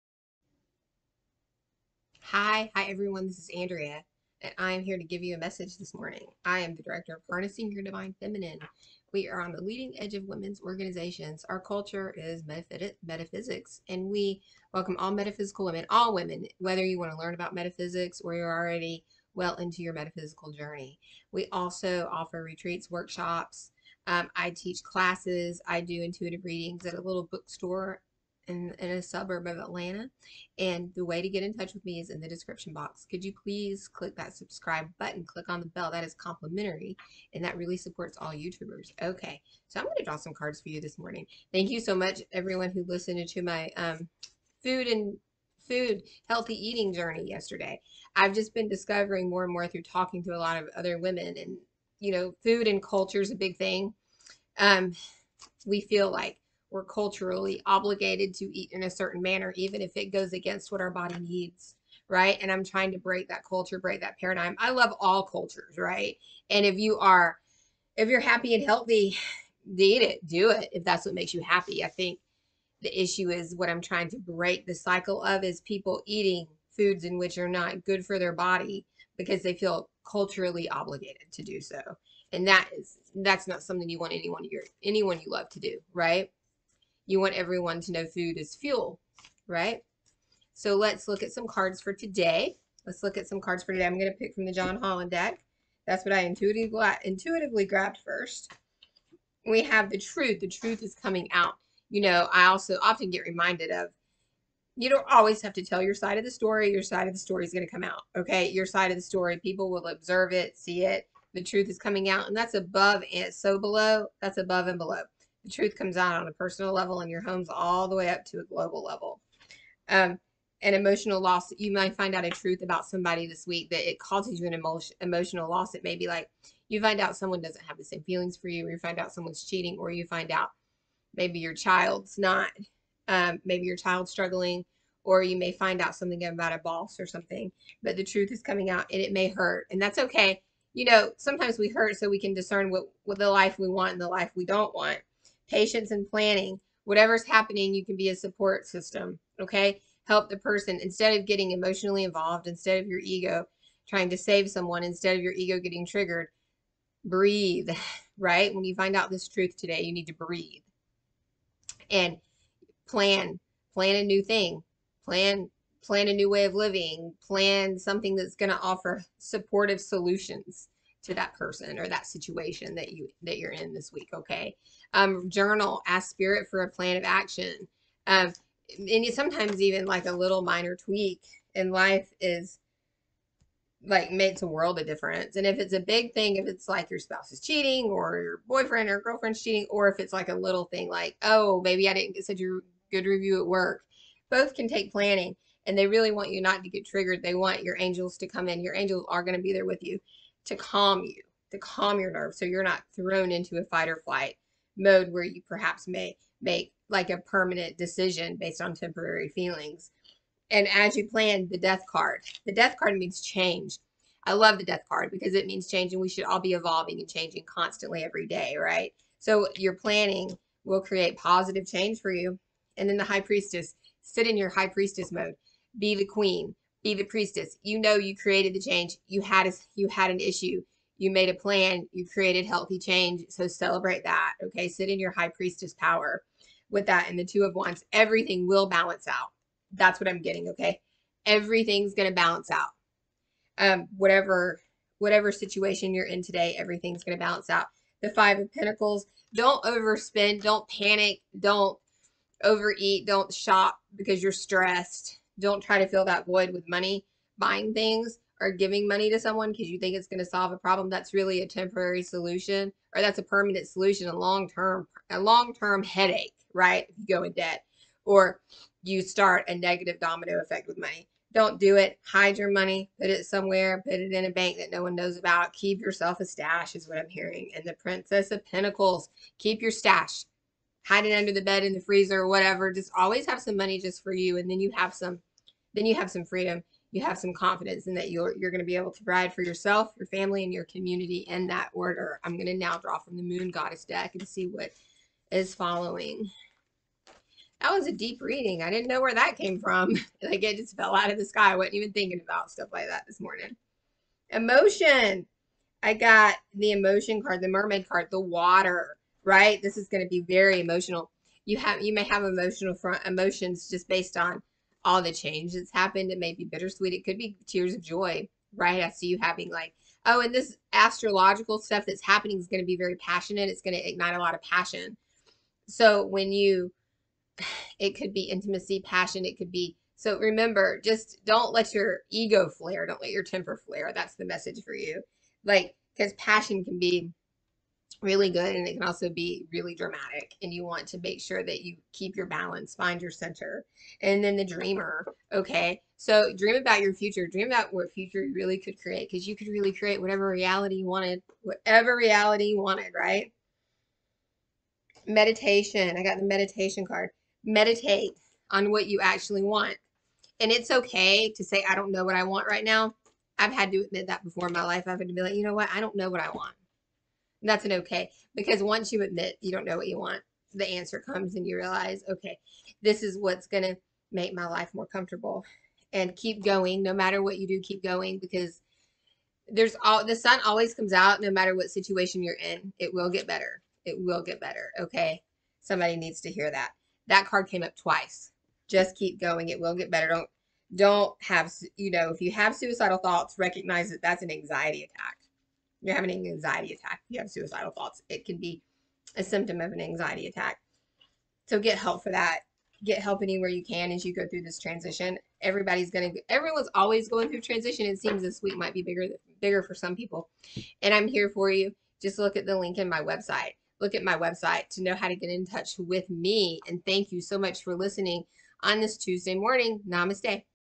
Please hold your ego in check.
Hold your temper and ego in check. As the truth comes out you want to creat a better life for yourself. w